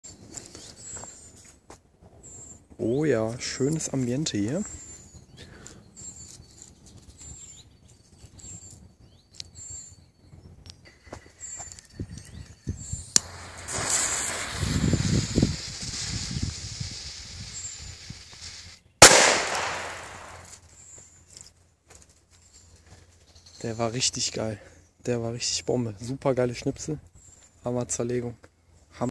China Böller D vom Feinsten -Comet- *WATCH* HD
Zerlegung und Knall: 1 glatt für heutige Verhältnisse.
Just wanted to share with you guys ;)